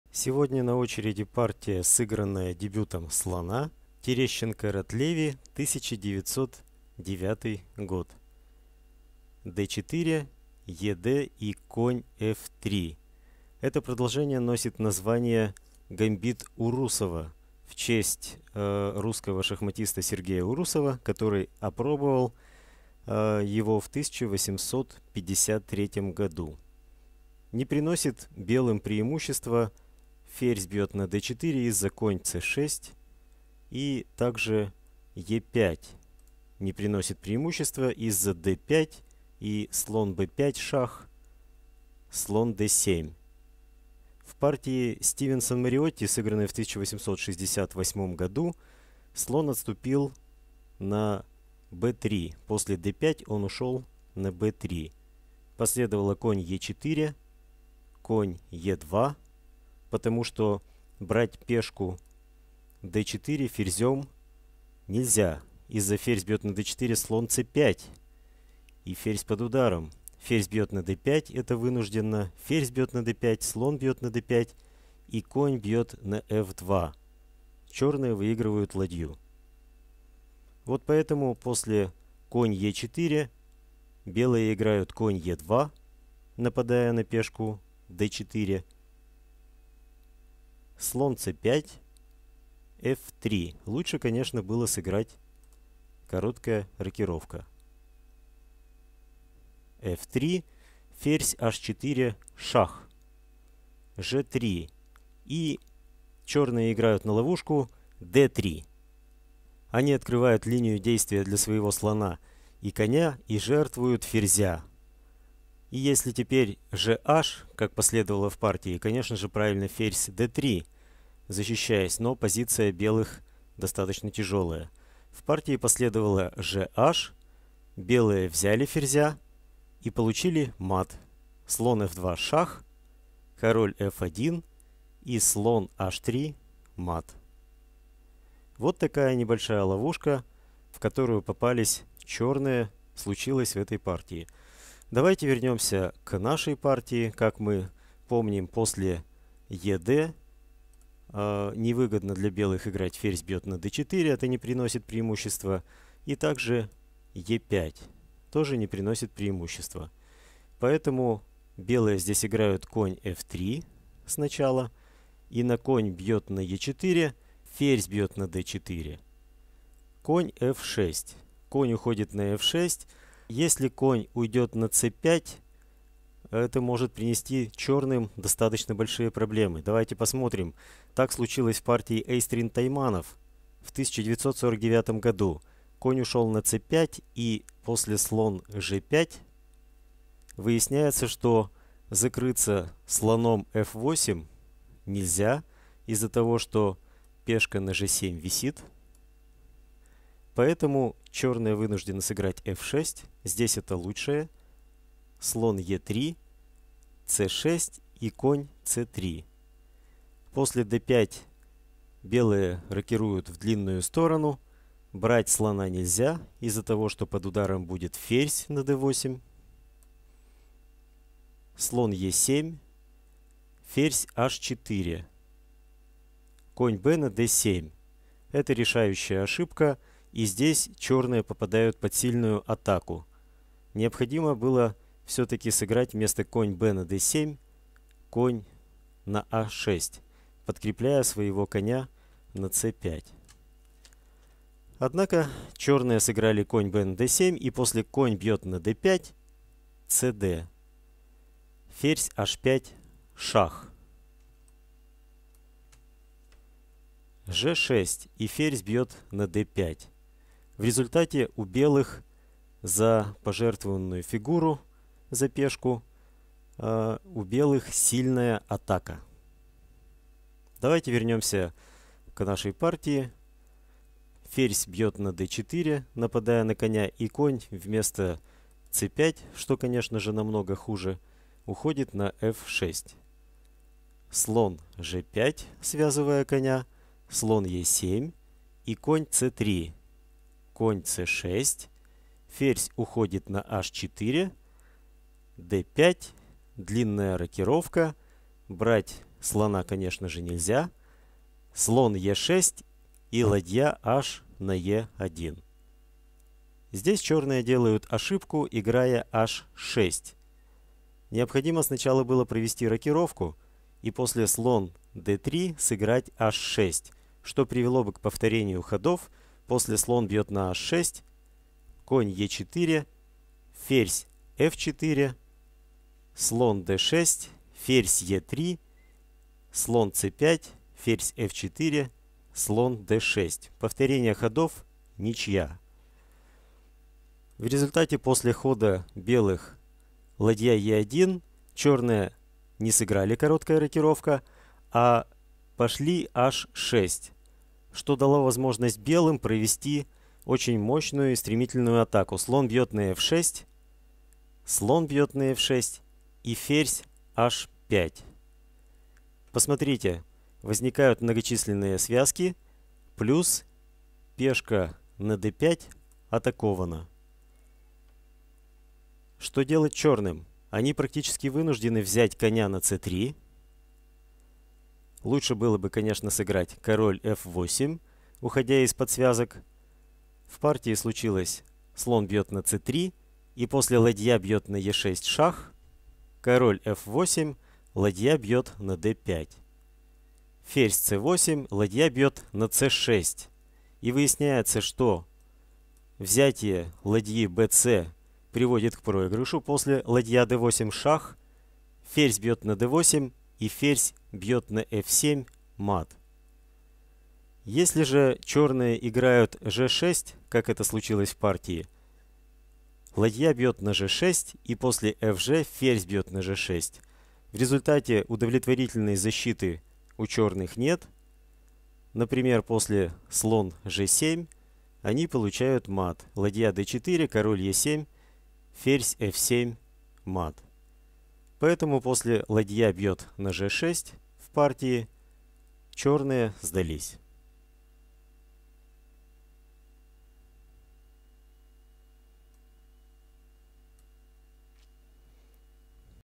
Шахматная ловушка и жертва Ферзя в Дебюте слона Гамбит Урусова!
Шахматная ловушка, в которую могут попасть белые в Дебюте слона (Гамбит Урусова). Красивая жертва ферзя! Жертва фигуры за атаку!